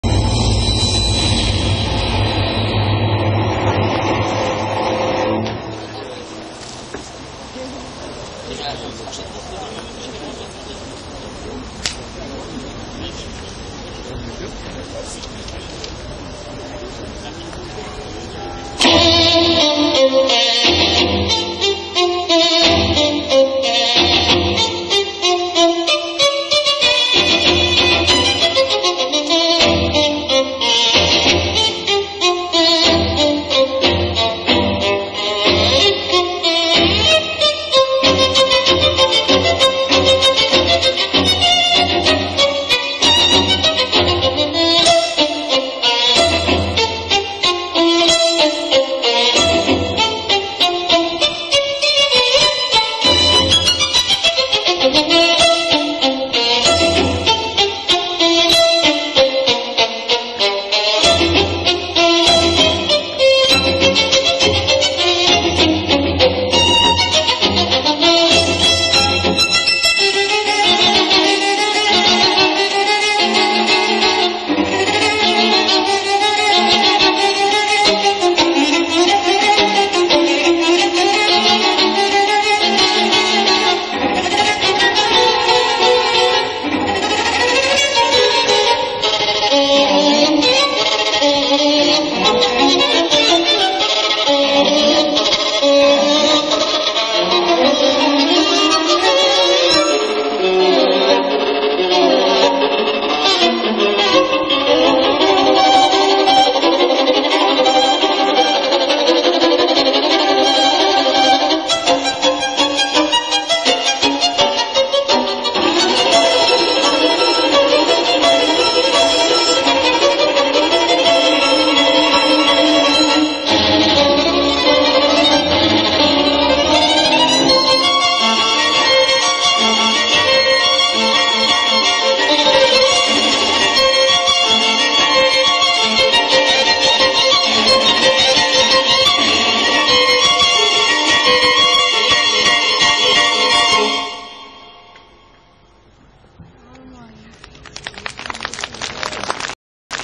День Фехтовальщика 2009 - кукла
Этот номер меня как-то не очень впечатлил. Хотя был по-своему довольно интересен. Ожившая кукла со шпагой в руках, похоже, пытающаяся понять, что она такое и зачем.